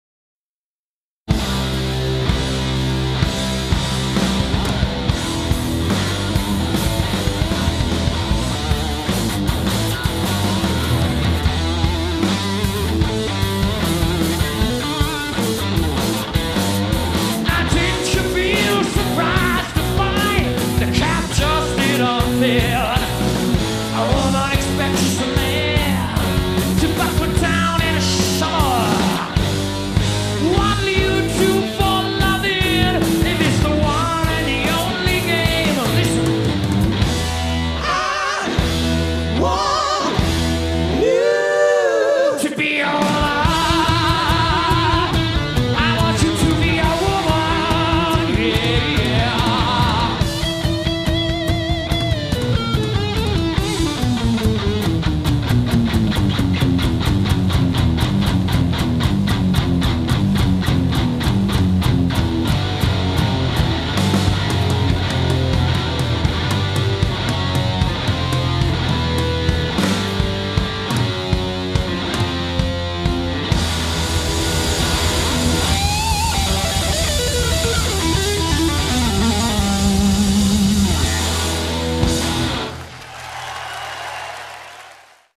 Queen - London 1975
Hammersmith Odeon Christmas Eve.
This song with new remastered audio was not included in the 2009 broadcast.